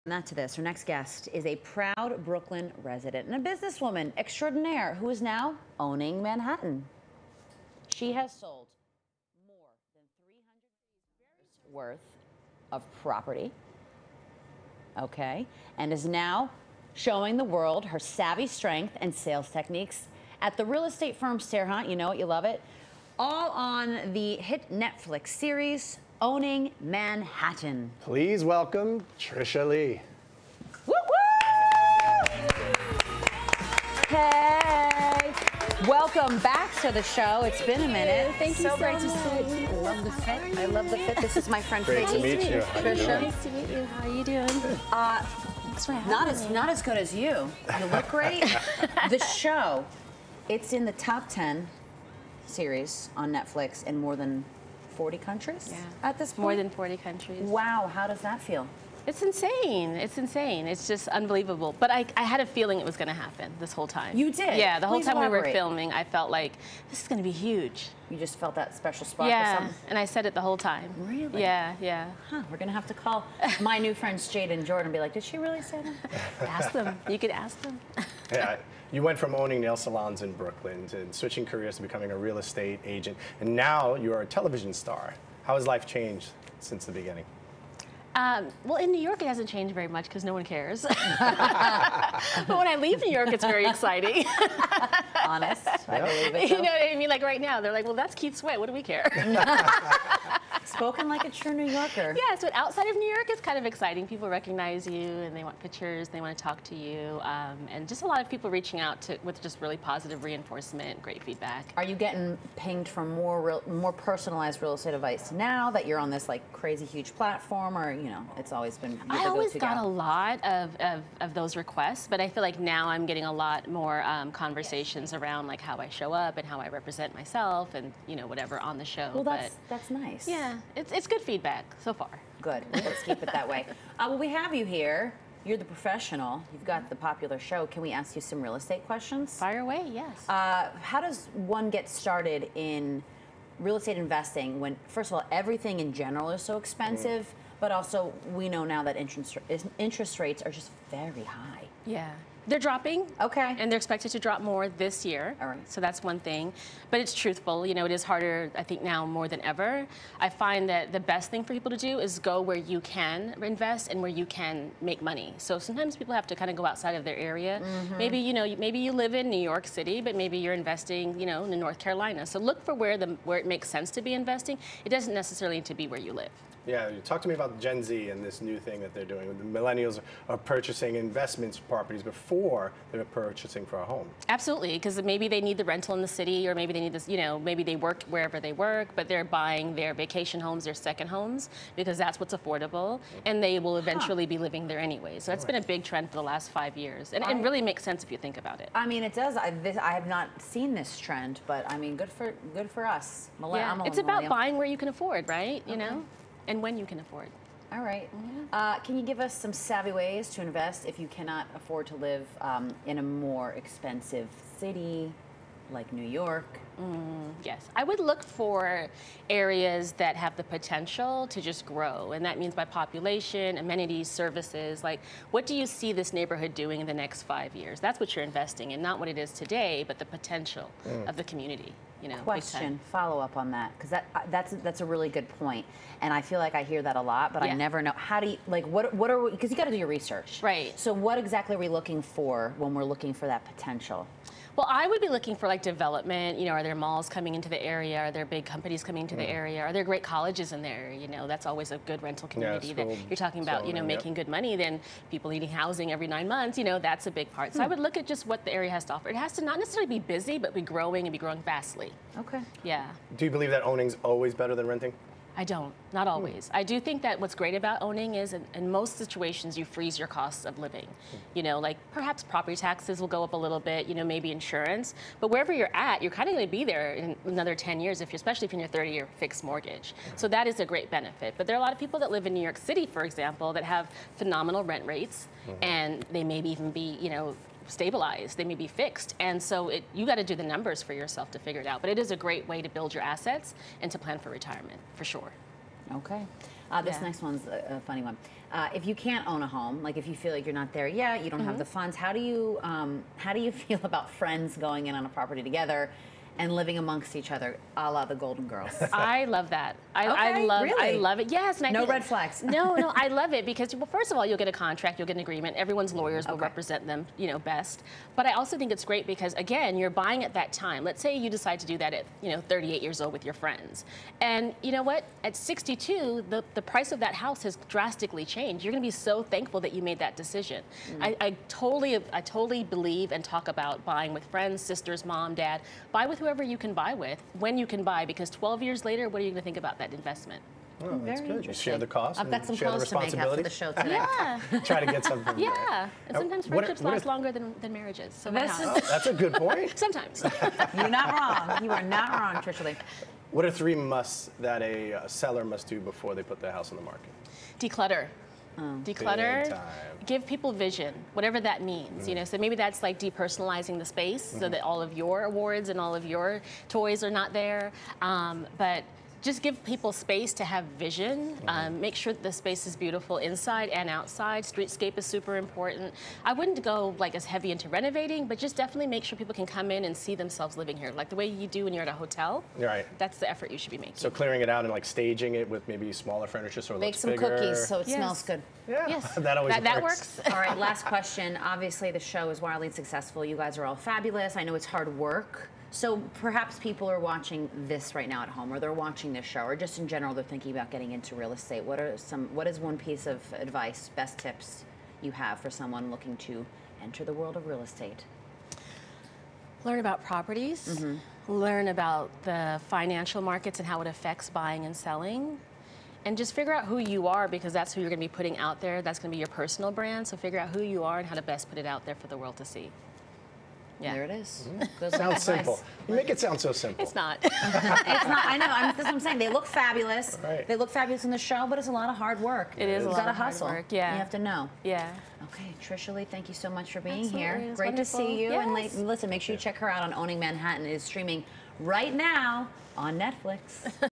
Real estate superstar Tricia Lee talks hit Netflix series “Owning Manhattan”
Tricia Lee gives real estate investment advice for home buying and shares the trends she sees among younger generations of buyers. She gives tips on what to look for when buying and discusses the pros of renting.